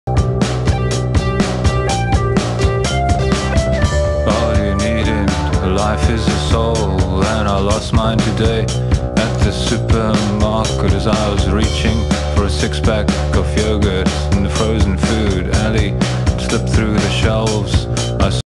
all you need in life is a soul
some characters i built, rigged and animated in 3D, mainly here for portfolio purposes.